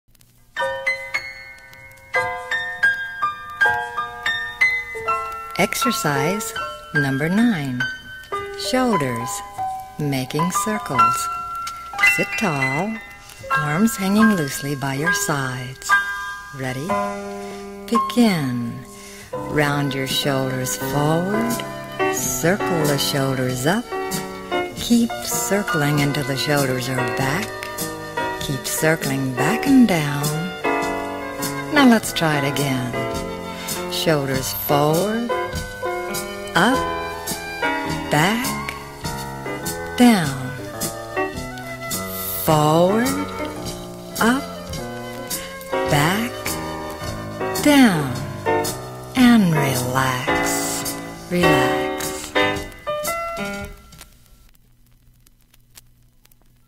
Exercise #9 - Manic Mark's Shape Up For The Spring Exercise Program
I'll be offering a series of 29 exercises designed to help us get in shape for Spring. Fit into that polka dot bikini or skimpy black Speedo!